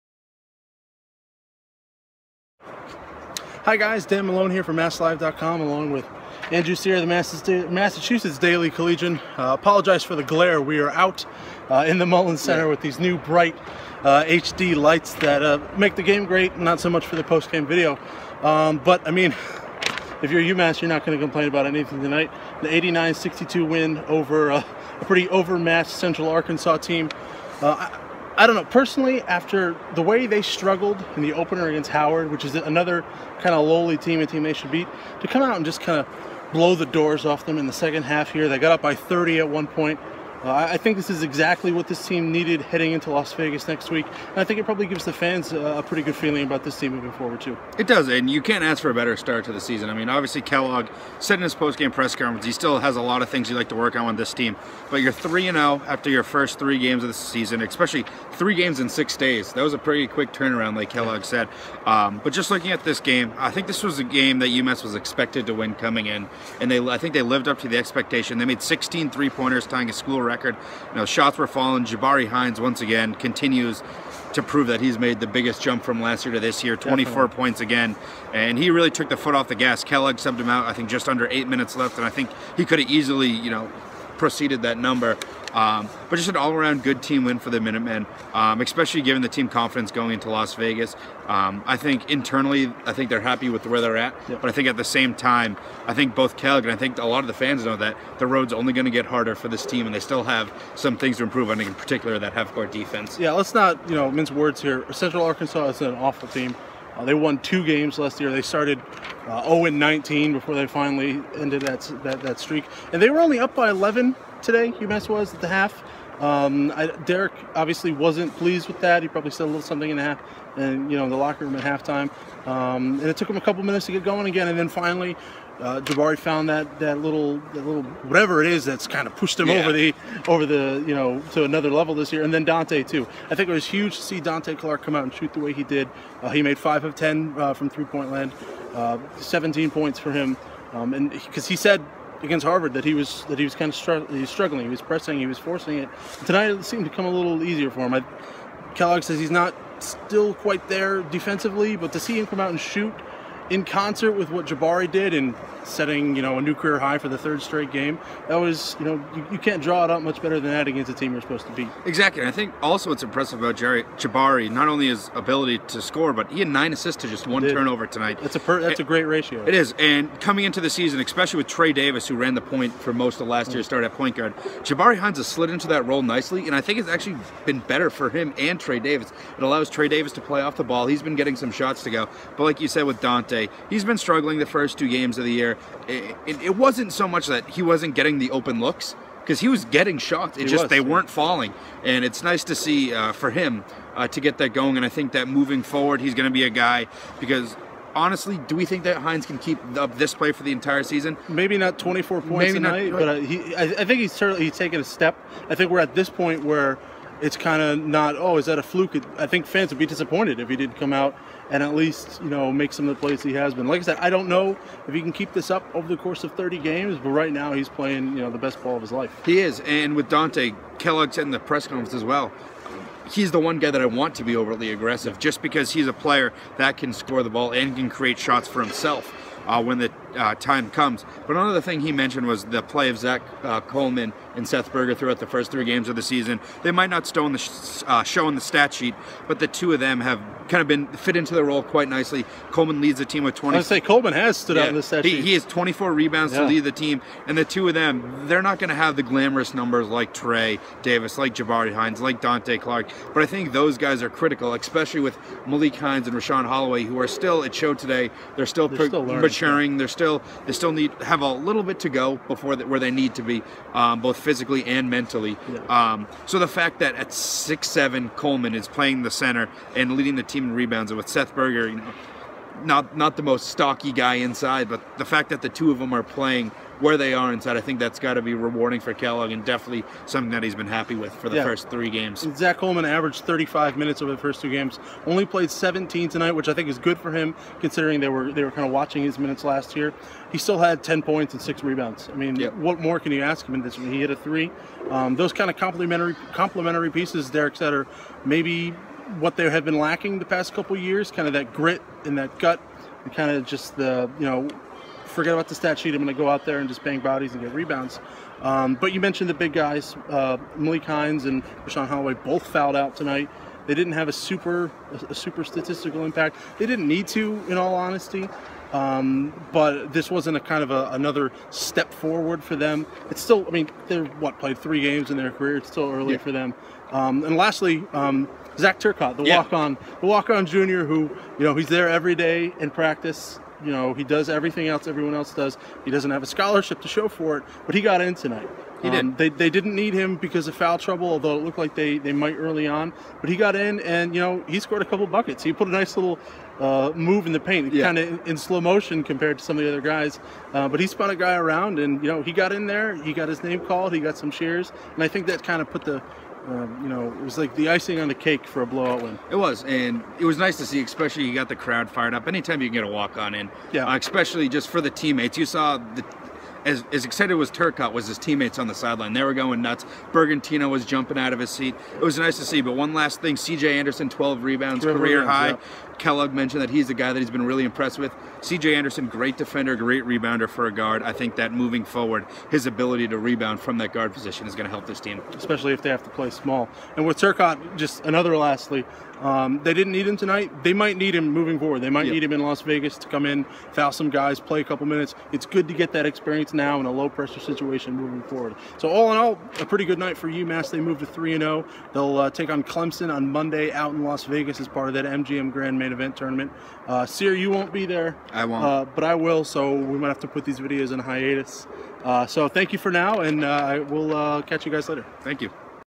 Postgame Analysis: Wrapping Up UMass' 89-62 Win Over Central Arkansas
Daniel Malone & Andrew Cyr share their thoughts on the Minutemen's big win Thursday night.